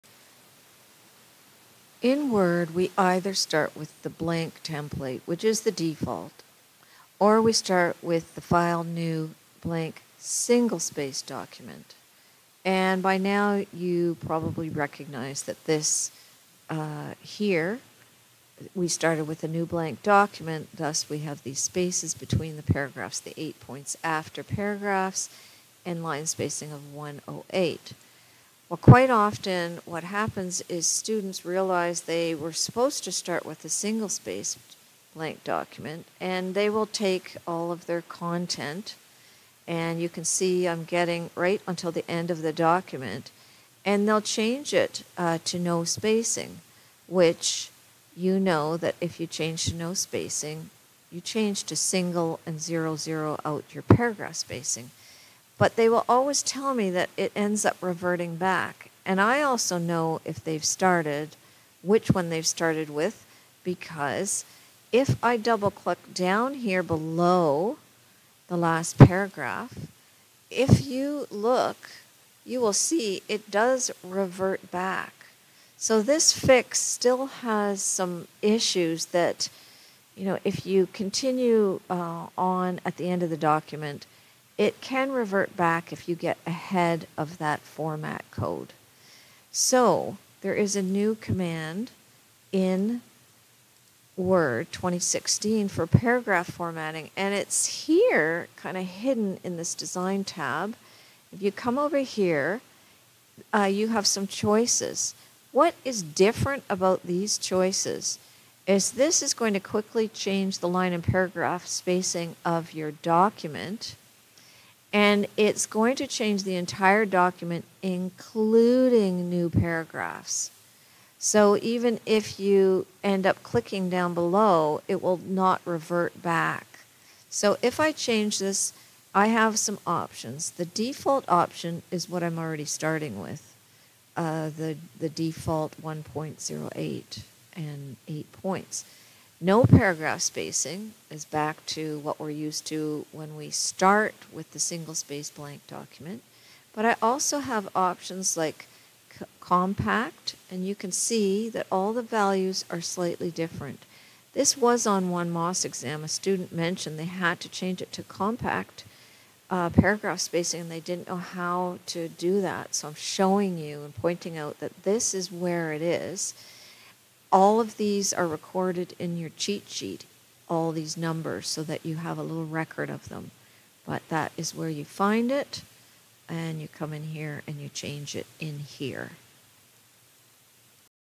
41 4 change Paragraph and Line Spacing in Design Tab
A fast way (two clicks) to change line and paragraph spacing for an entire document without selecting any text. The settings will NOT revert back to the template you started with.